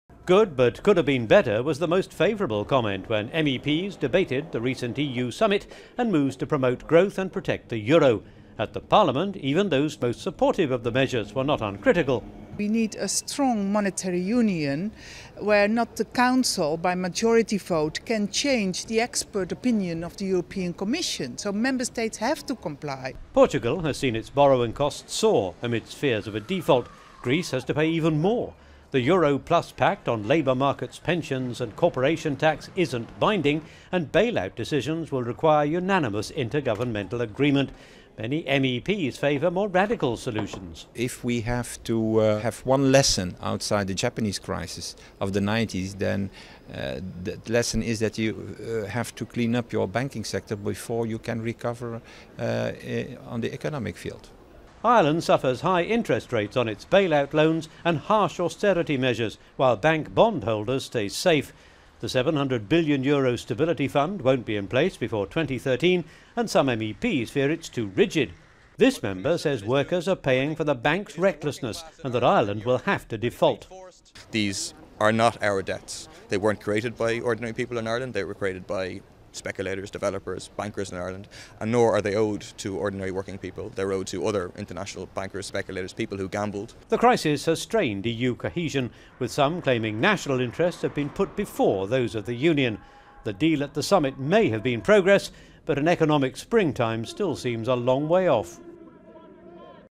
Save the euro
MEPs welcome EU summit conclusions to promote growth and save the euro -but would have preferred something far tougher.